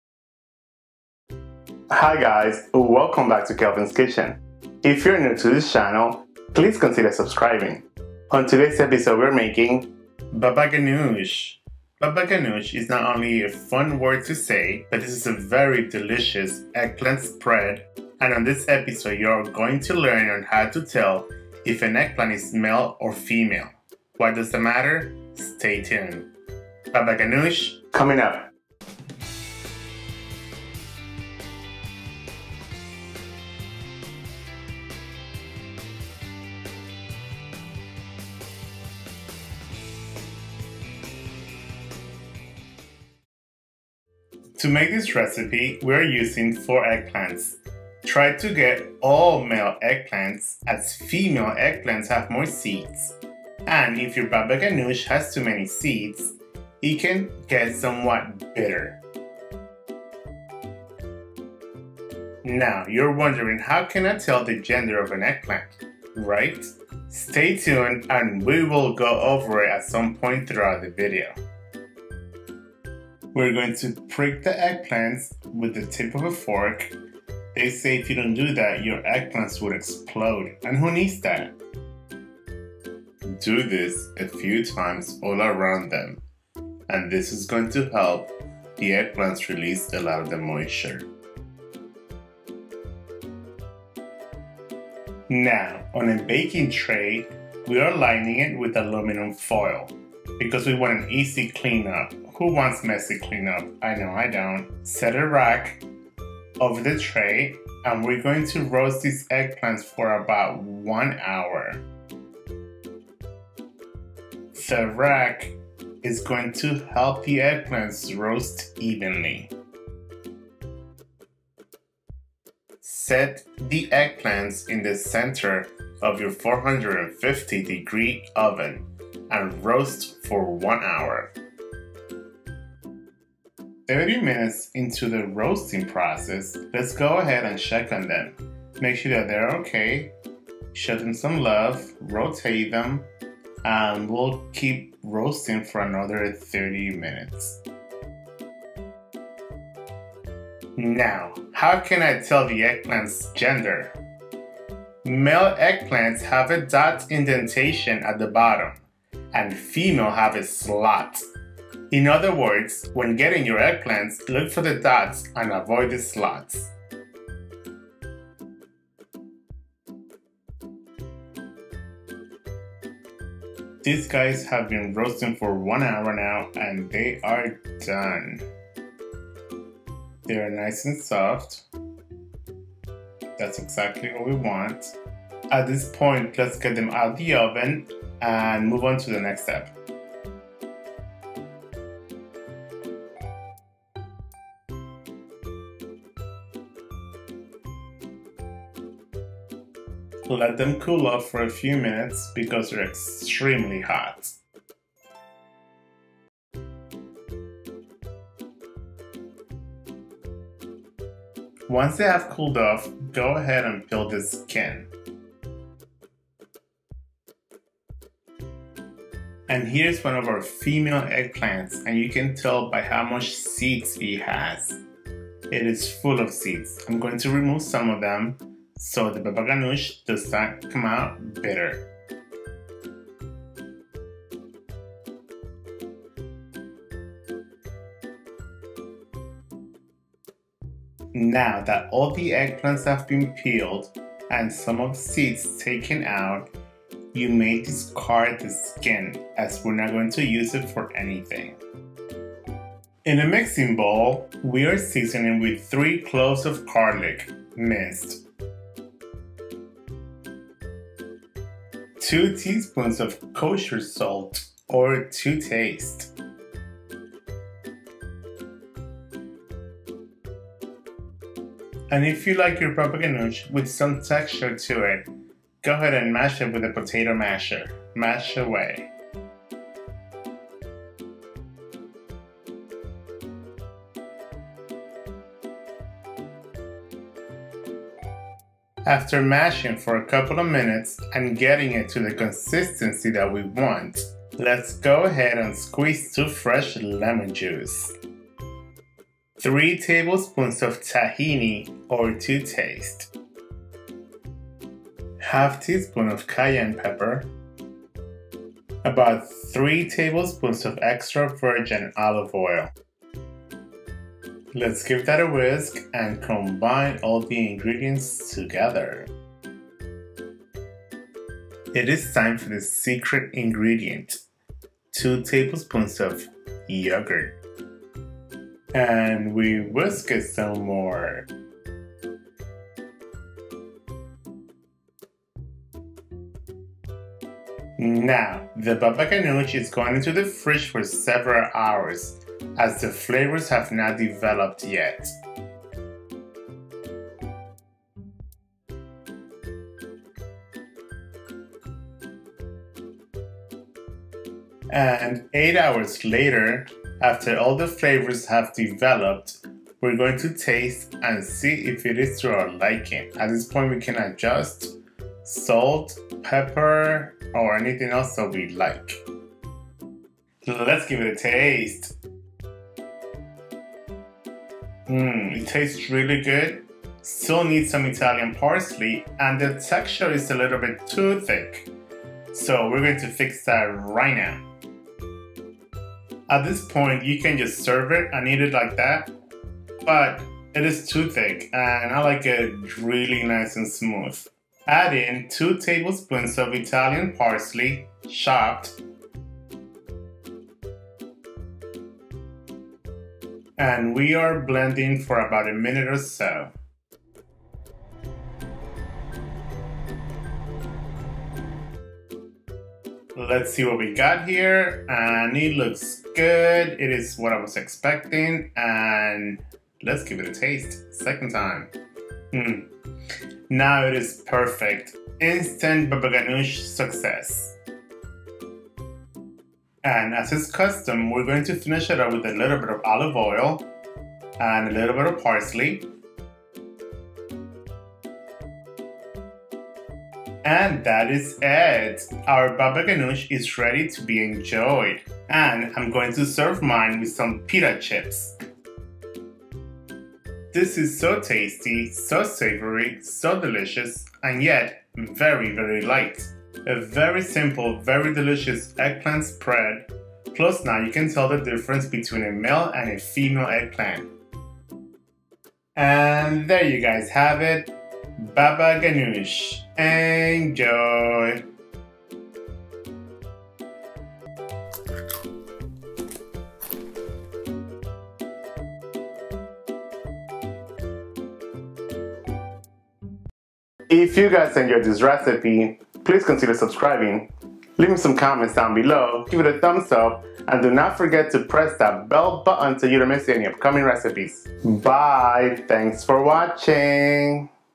How to Make Roasted Eggplant Dip & Spread Baba Ganoush
Baba Ganoush - How to Make Roasted Eggplant Dip & Spread

Ingredients for about 6 cups Baba Ghanoush:
3 or 4 large Italian eggplant
2 cloves crushed garlic
2 tsp kosher salt, or to taste
juice from 1 or 2 lemons, or to taste
3 tbsp tahini, or to taste
pinch of cayenne
3 tbsp extra virgin olive oil
2 tbsp plain Greek yogurt
2 tbsp chopped fresh Italian parsley leaves
pita chips and vegetables for dipping

Directions:
Pre-heat over at 450 degrees and roast for 1 hour
Turn frequently with tongs while skin chars. Cook until eggplants have collapsed and are very soft, about 1 hour. Transfer to a bowl and cover tightly with aluminum foil and allow to cool, about 15 minutes.
When eggplants are cool enough to handle, split them in half and scrape flesh into a colander placed over a bowl. Drain 5 or 10 minutes.
Transfer eggplant to mixing bowl. Add crushed garlic and salt; mash until creamy but with a little texture, about 5 minutes. Whisk in lemon juice, tahini, olive oil, and cayenne pepper. Stir in yogurt.
Cover bowl with plastic wrap and refrigerate until completely chilled, 3 or 4 hours. Taste to adjust seasonings. Stir in parsley.

Pita Chips

Filming Equipment:
Canon 80D
Canon M50
Canon Macro Lens
SD Memory Cards
Lavalier Mic
USB: Mic
Over Head Tripod